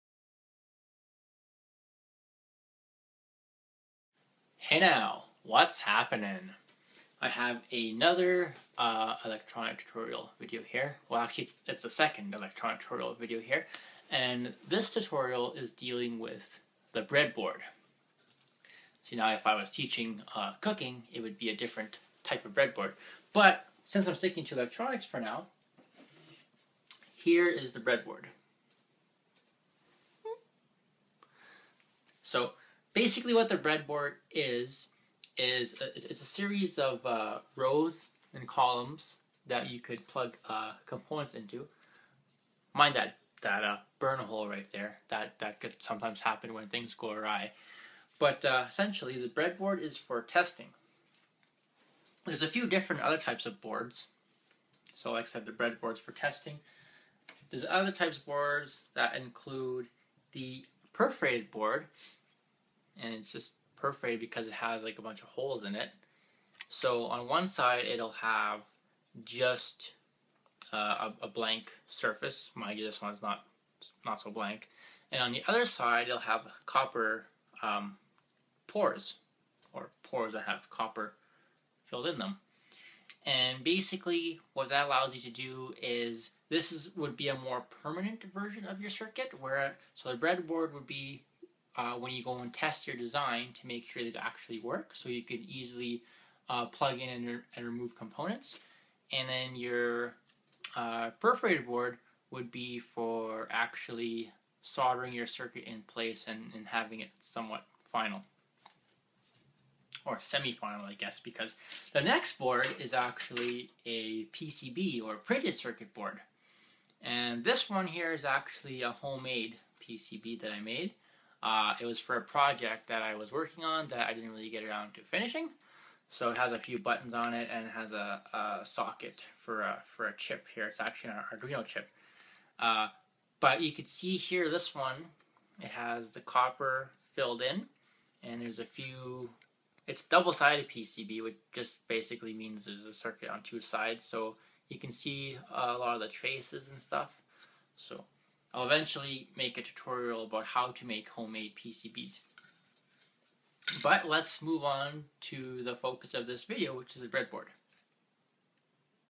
Beginner Electronics Tutorials - The Breadboard
This is the second video in the series of Basic Electronics Tutorials. This tutorial covers the Breadboard. Which is essentially a prototyping board.